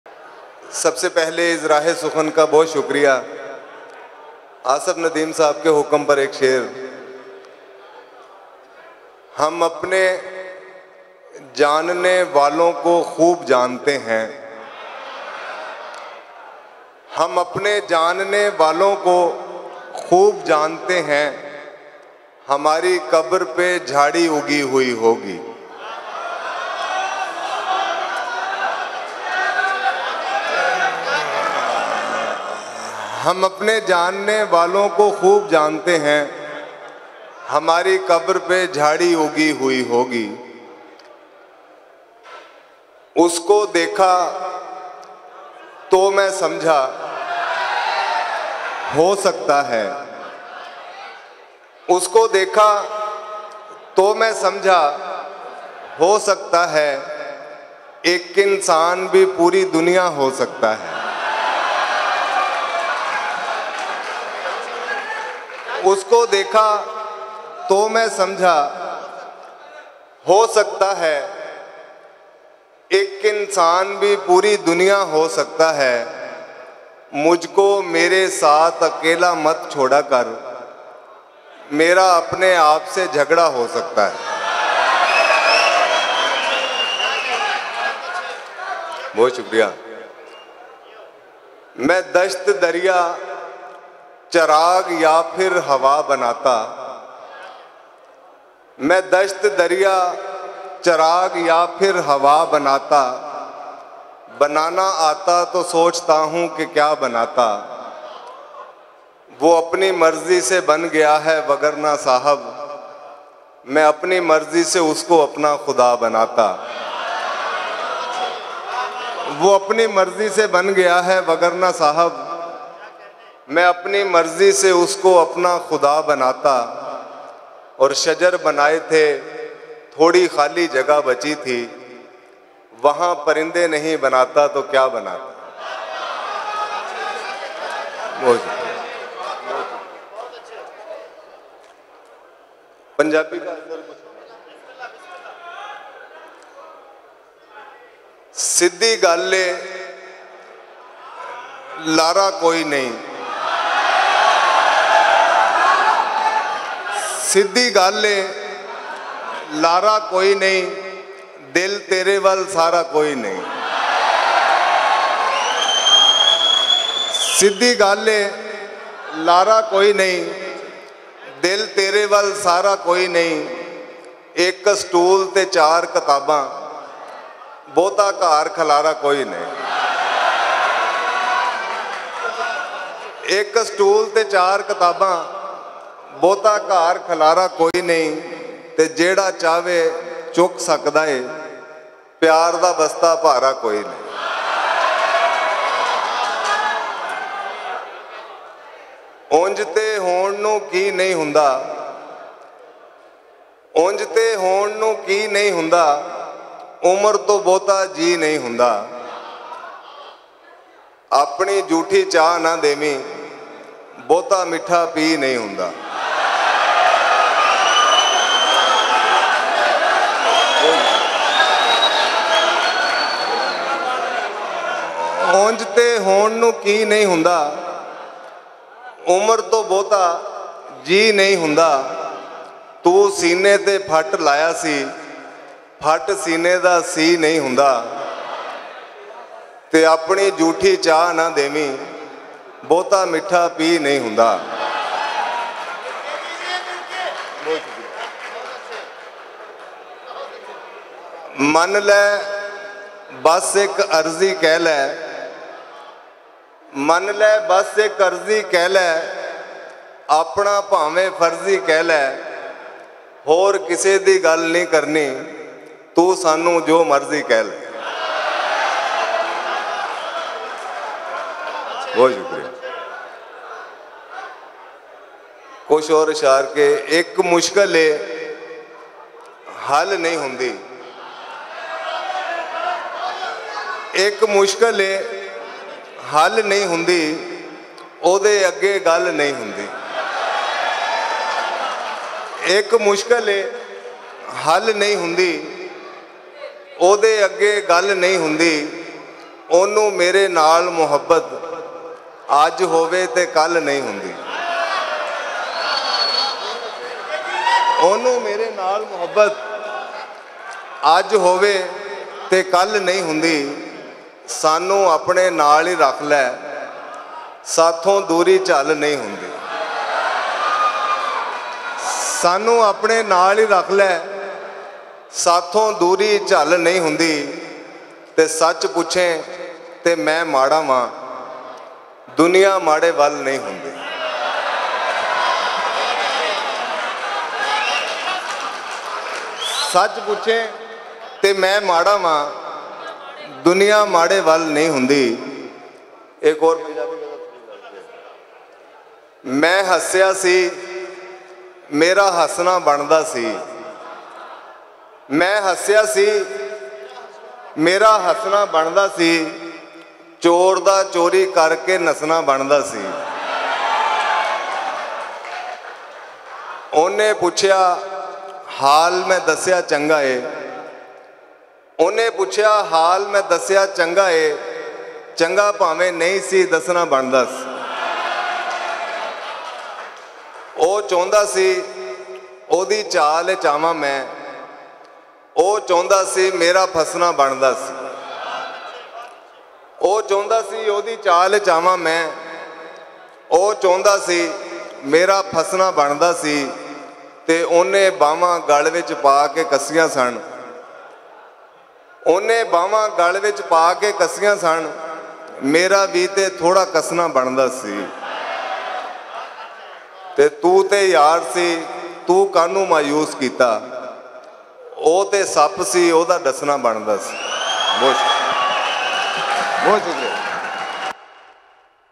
Mirza Faisal | Azrah e Sukhan Mushaira 2024 | Mian Channu | Latest Urdu Poetry
Watch Mirza Faisal , famous Urdu & Punjabi Poet reciting his best poetry in Mushaira held by Team Azrah e Sukhan.

In the present era, we are bringing a series of literary sessions under the platform of "Azrah-e-Sukhan" for the cultural vitality of Urdu and Punjabi poetry.